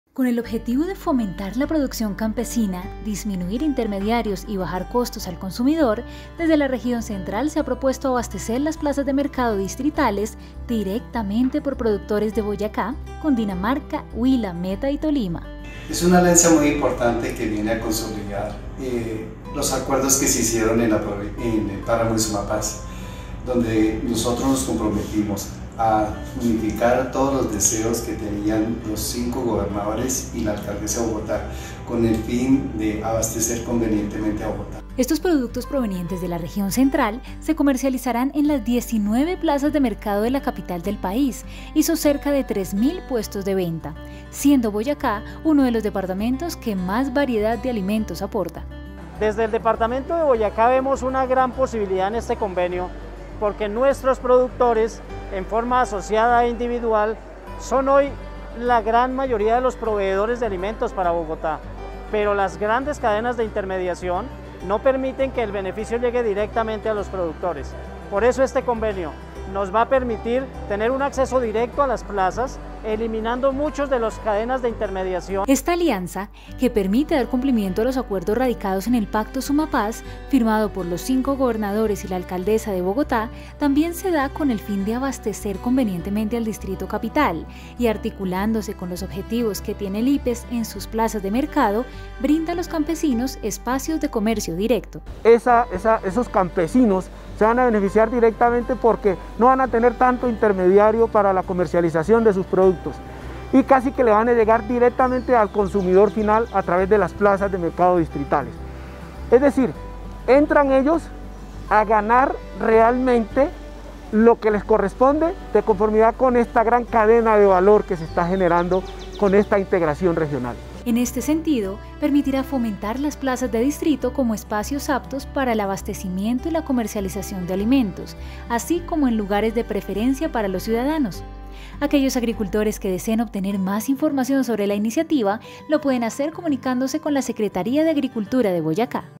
BOYACENSES PROVEEN PLAZAS DE MERCADO DISTRITALES
Las plazas de mercado distritales serán abastecidas directamente por productores boyacenses y de departamentos como Cundinamarca, Huila, Meta y Tolima, pues comercializarán sus productos en las 19 plazas de mercado de la Capital del país y sus cerca de 3.000 puestos de venta.